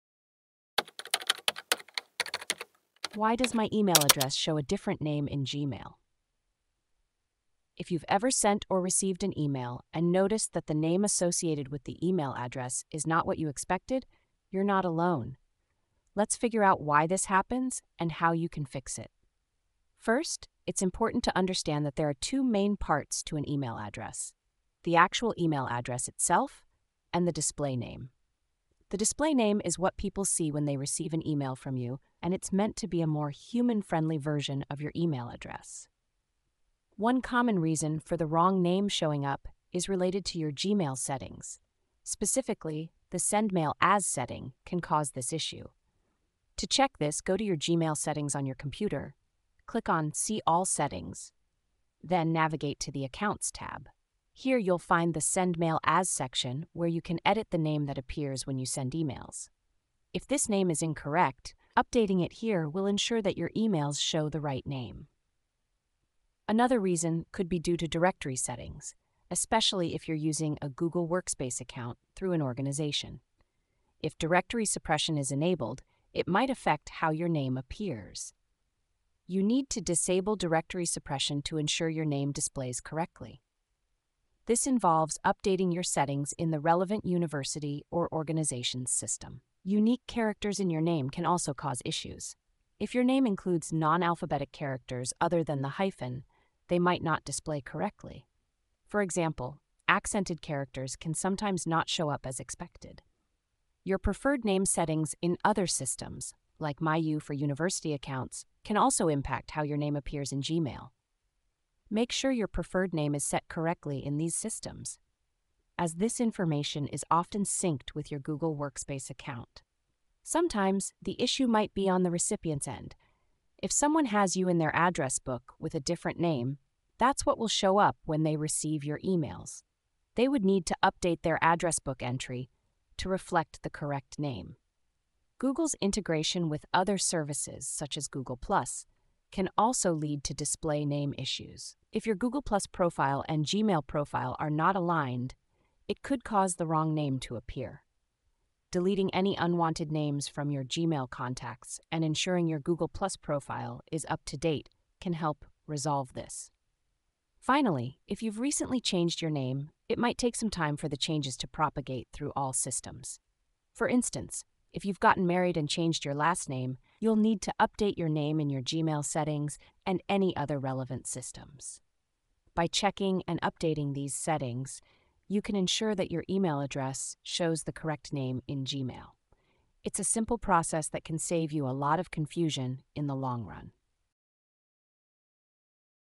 Why Does My Email Address Show A Different Name Gmail? - TheEmailToolbox.com
Have you ever noticed that the name displayed in your Gmail account isn’t what you expected? The display name is what recipients see when they get an email from you, and it should reflect your identity accurately.

We'll walk you through the steps to check and update your Gmail settings, including the "Send mail as" feature, which can often lead to confusion. If you're part of an organization using Google Workspace, we’ll discuss how directory settings might affect your display name. Additionally, we’ll highlight how unique characters in your name or settings in other systems, like university accounts, can impact what people see.

If you’ve recently changed your name, we’ll explain how to ensure that all your settings are updated accordingly. We’ll also touch on how the recipient’s address book can influence the name displayed when you send emails.

Join us as we provide practical solutions to these common email display issues.